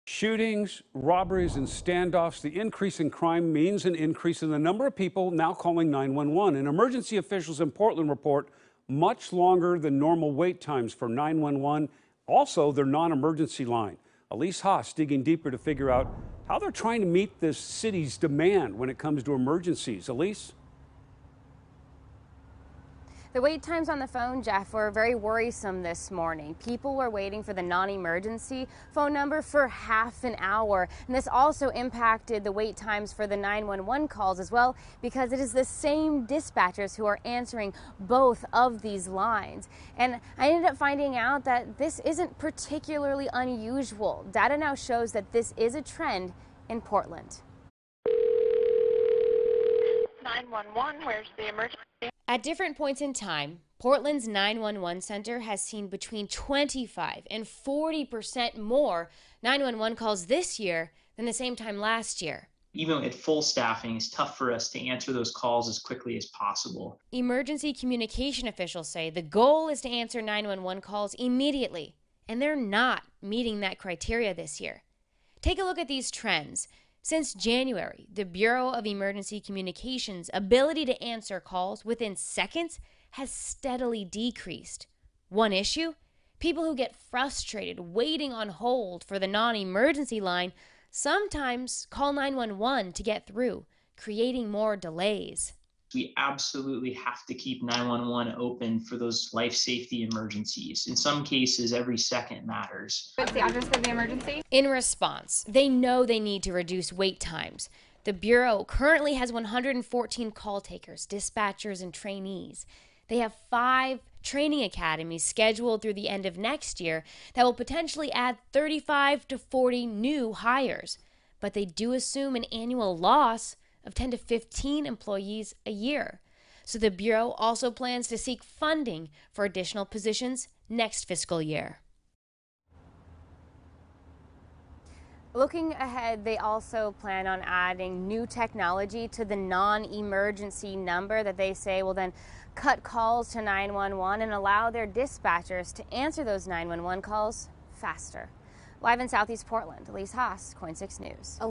Portland 911 struggling to answer calls 'as quickly as possible'
Emergency officials reported longer-than-normal wait times for 911 and non-emergency callers on Thursday as law enforcement officials responded to multiple incidents across Portland.